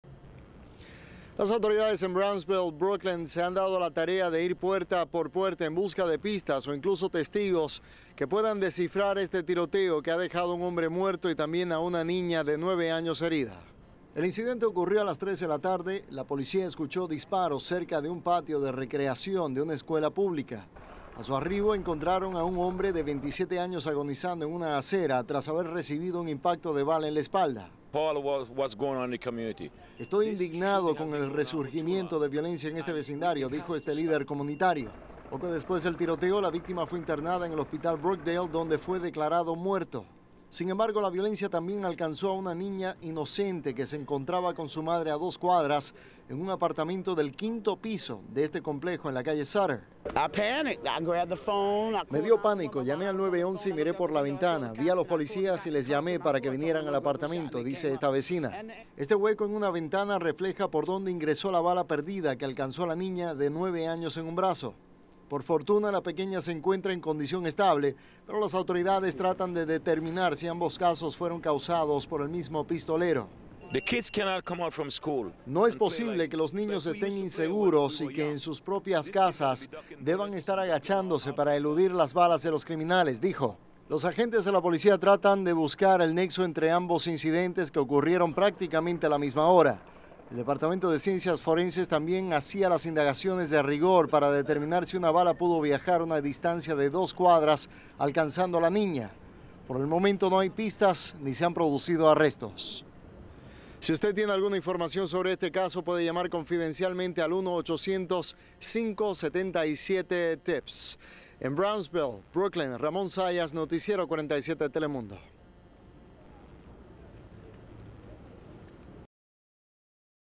Daily News PKG - Shooting in Brooklyn NY
Little girl gets injured in a Brooklyn shooting. Shot and Edit